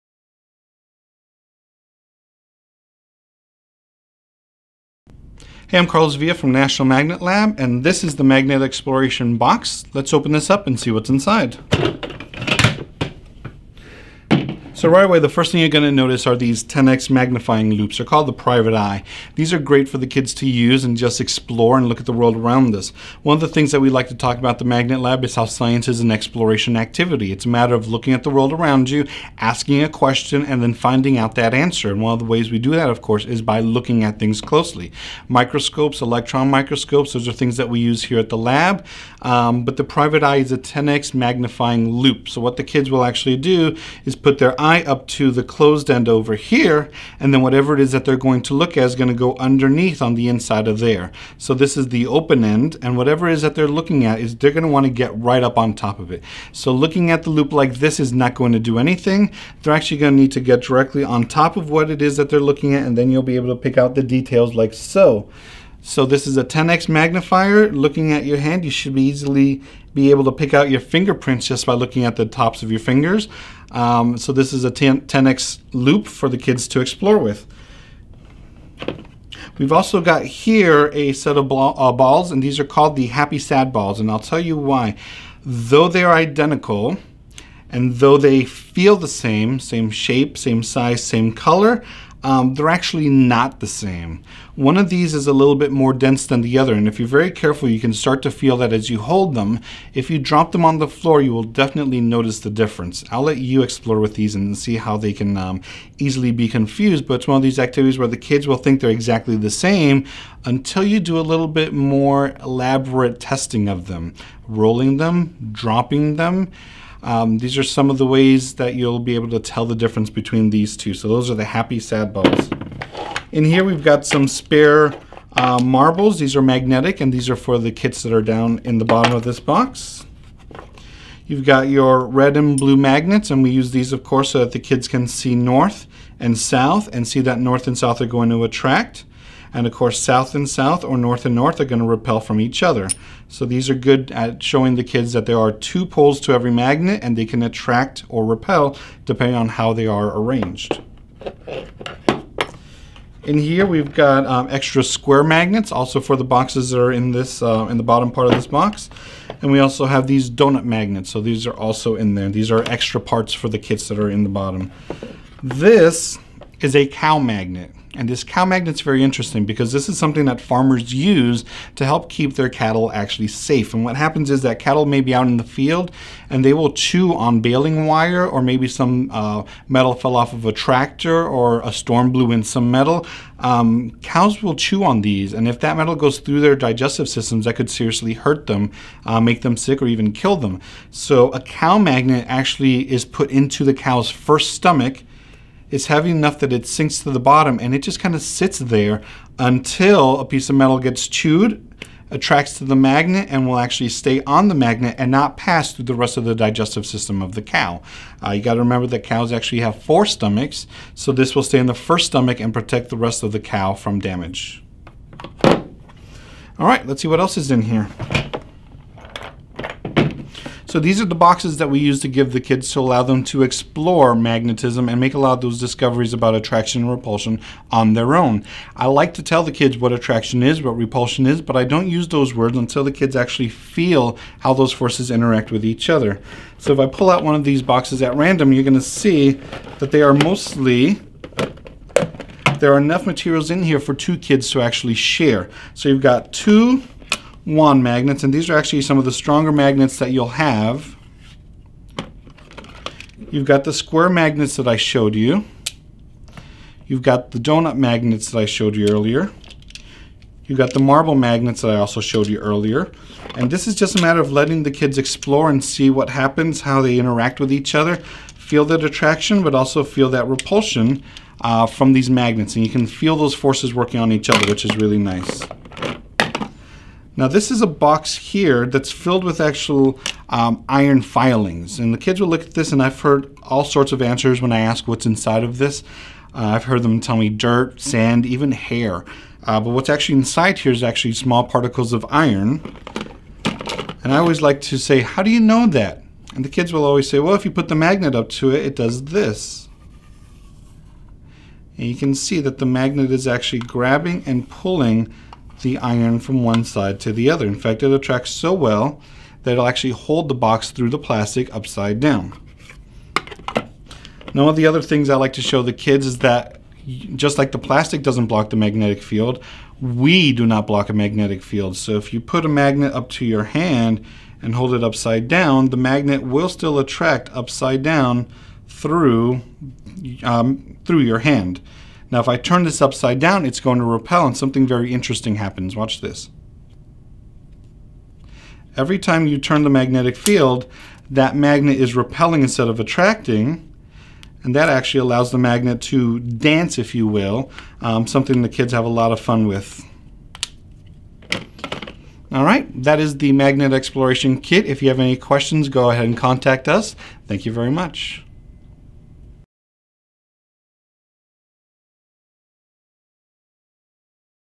Classroom Kit: Magnet Exploration
In this classroom kit, you find materials to help explain how magnets work.